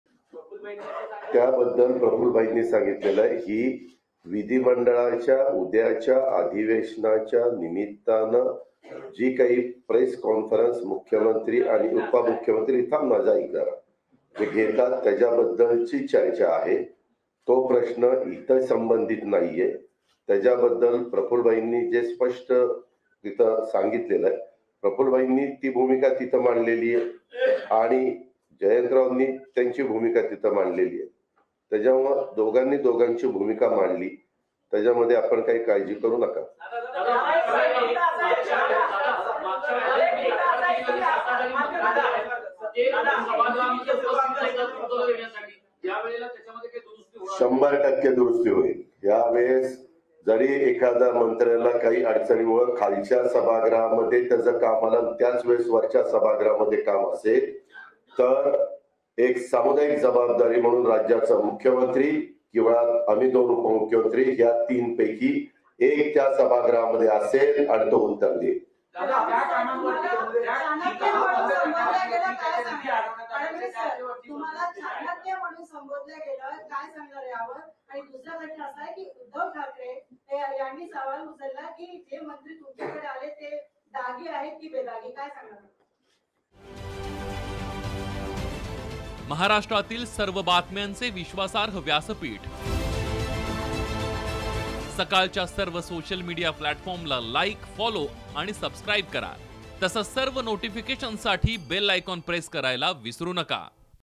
Ajit Pawar Sharad Pawar भेटीत काय घडलं ?, दादांनी दिलं उत्तर | NCP CRISIS
अधिवेशन पूर्वी महायूती सरकारची पत्रकार परिषद, सरकारने केलेल्या कामाचा पाढा वाचत विरोधकांना टोले लागावले.